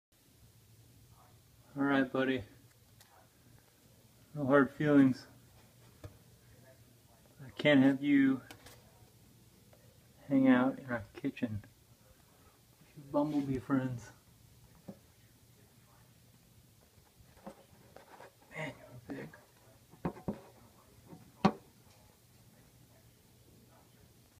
Bee Adventure: A Curious Bumblebee Visits My Kitchen!
This is a video of a bumblebee I caught in my kitchen in a glass cup. I released it shortly after. Just a video of a bee trapped in a cup! No bees were harmed here. Enjoy this bee content.